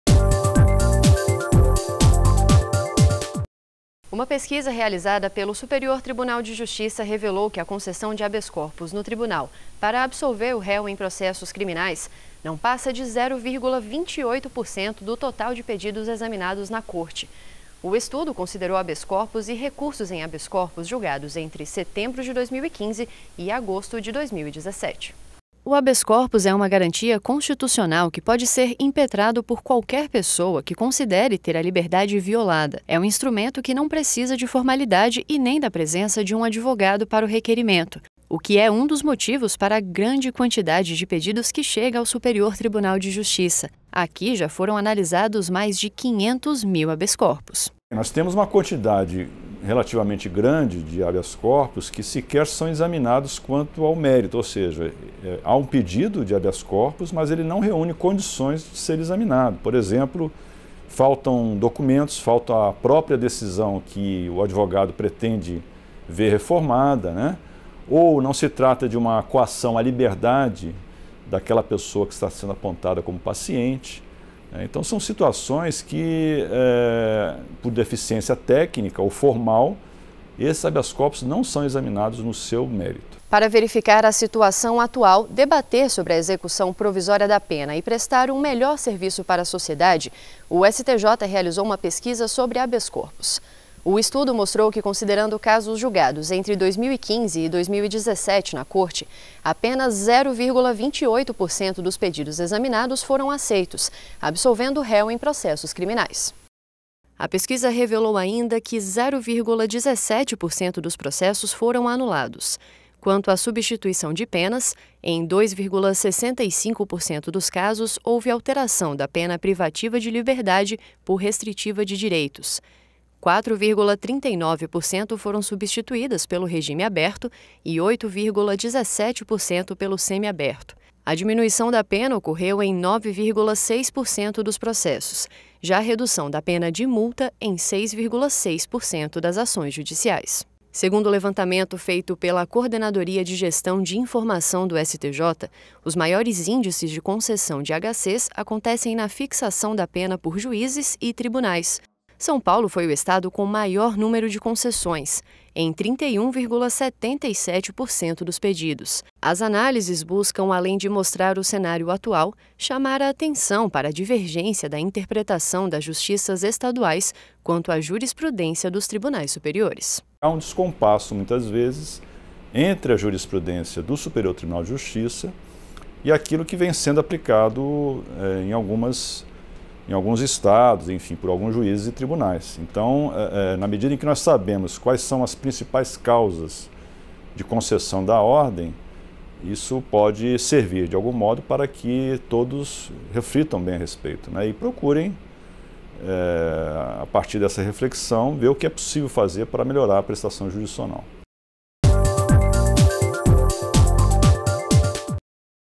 Absolvição por meio de habeas corpus é pequena, revela pesquisa do STJ (07/08/2019)
Uma pesquisa realizada pelo Superior Tribunal de Justiça revelou que a concessão de habeas corpus no Tribunal, para absolver o réu em processos criminais, não passa de 0,28% do total de pedidos examinados na Corte. O estudo considerou os habeas corpus e recursos em habeas corpus julgados entre setembro de 2015 e agosto de 2017.